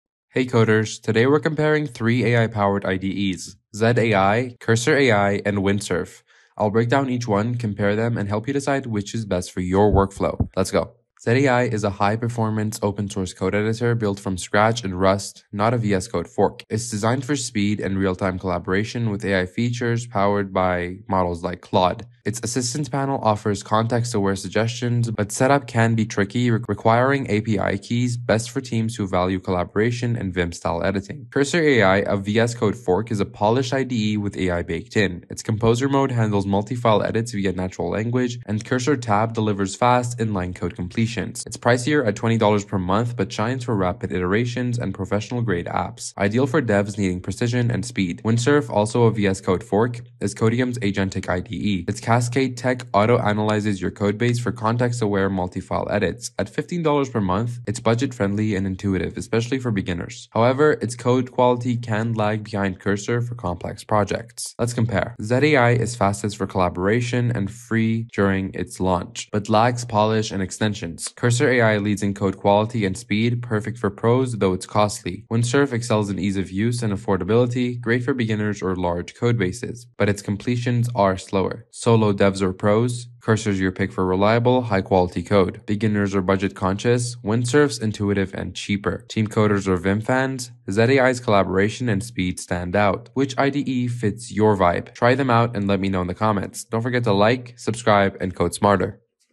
Zed Ai vs Cursor Ai vs Windsurf | Which AI Coding Tool is BETTER in 2025?
In this video, we compare Zed Ai's predictive modeling, Cursor Ai's data analysis features, and Windsurf's natural language processing. We break down the strengths and differences of these top 2025 AI platforms to help you decide which software is the best fit for your AI needs. Tune in for an in-depth look into the future of Artificial Intelligence!